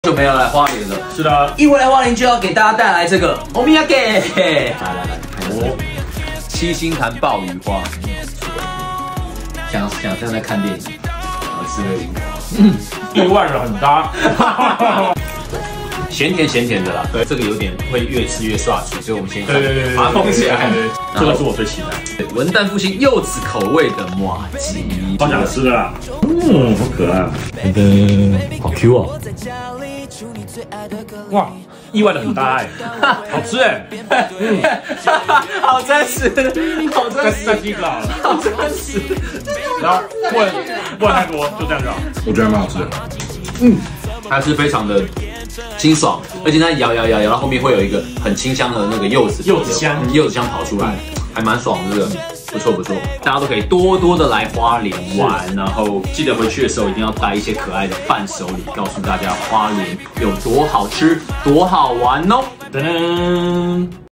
GX 鼓鼓 呂思緯 蕭秉治 大啖花蓮美食 超好吃伴手禮推薦
❚ 三立電視最新節目資訊➟ 戲劇 綜藝 實境 一次滿足✔️
❚ 大型直播➟ 金鐘獎 跨年演唱會 夏戀嘉年華 線上直播✔️